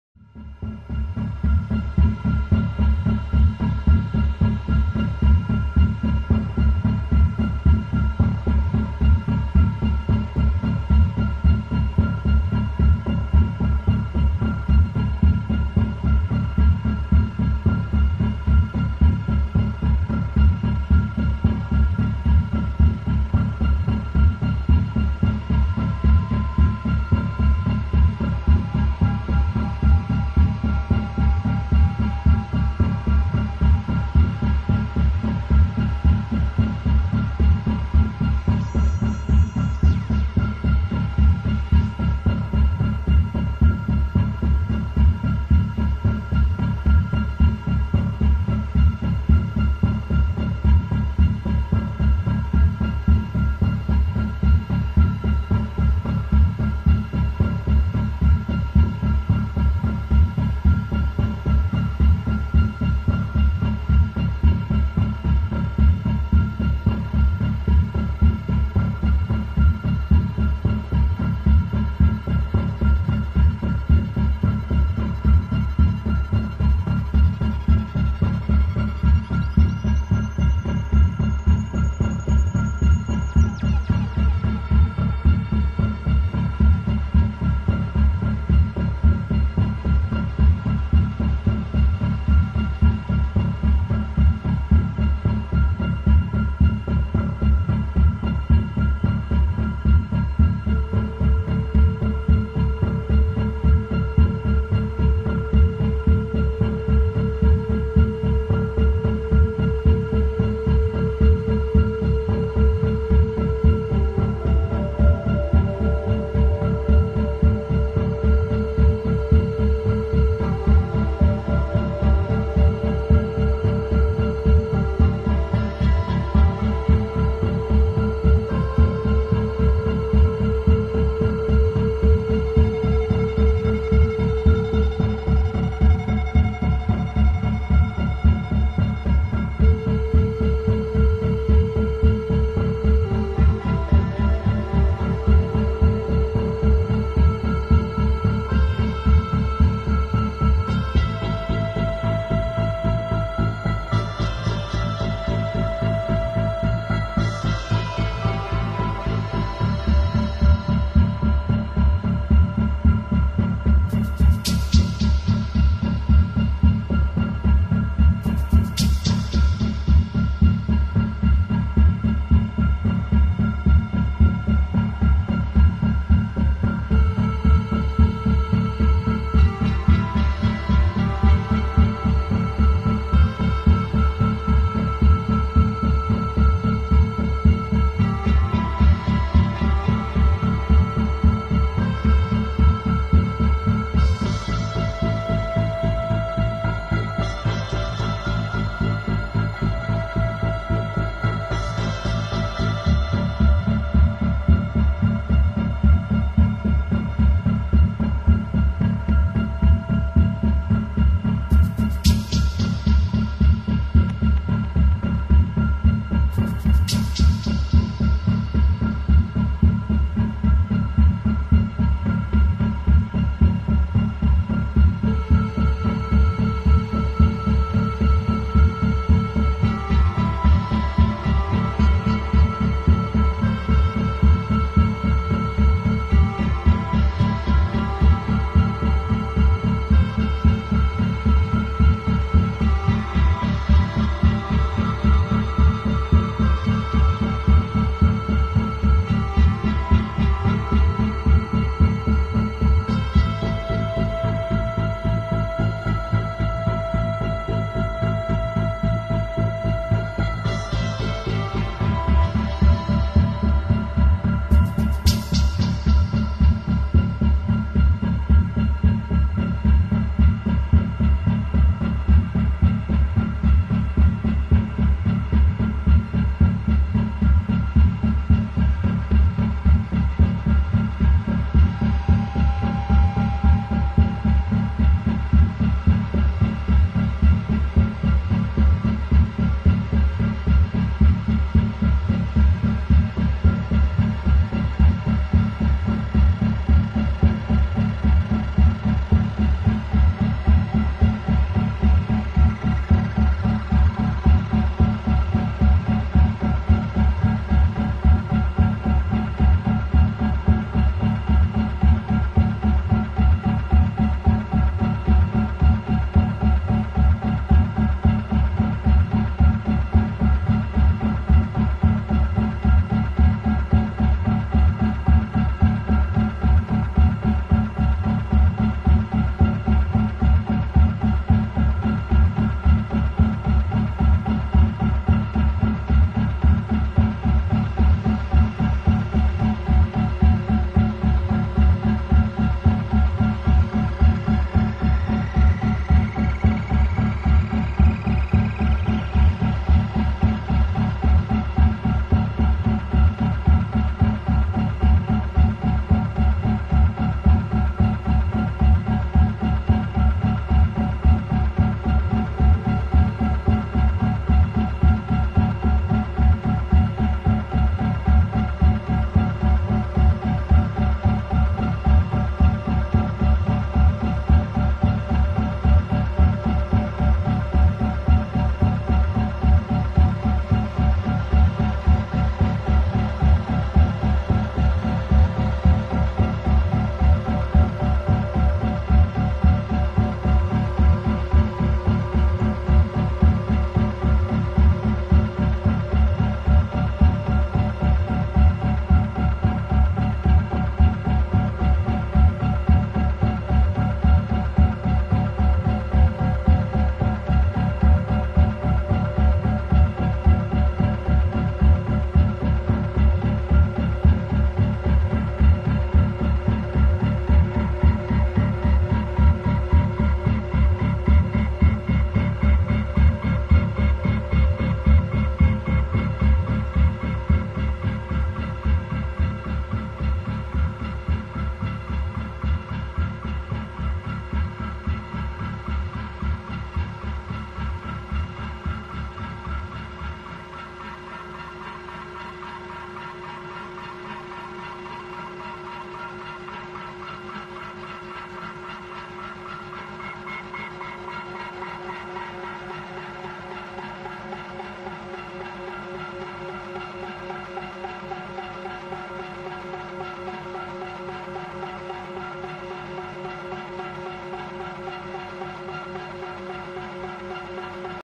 2010-09-22 synth-session.
A sort of continuation from my previous session. This time i added some  Grendel Drone Commander noise.